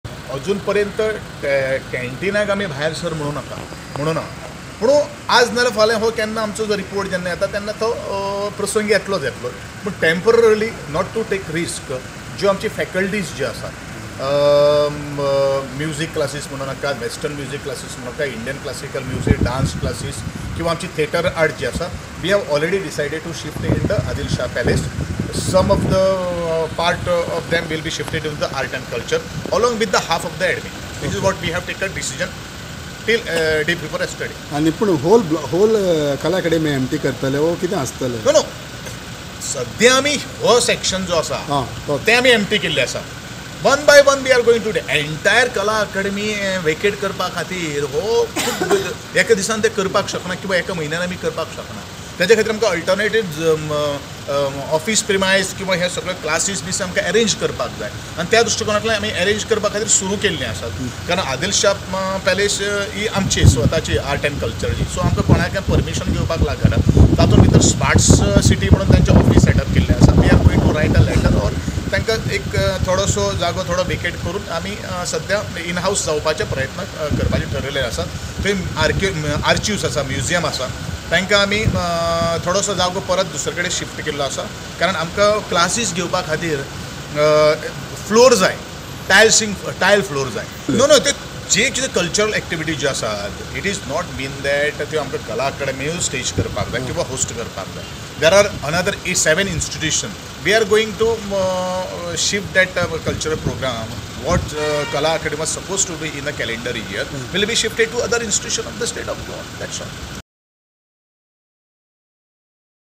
SHIFTING DANCE AND MUSIC CLASSES FROM KALA ACADEMY TO ADIL SHAH PALACE
The government is contemplating closing down the Kala Academy complex at the earliest, for its renovation and up-gradation and further, shifting the dance and music classes conducted in this premier art and culture institution to Adil Shah Palace, till the completion of the said repair work. Art and culture minister Govind Gaude said that music classes and canteen will be shifted in phase manner.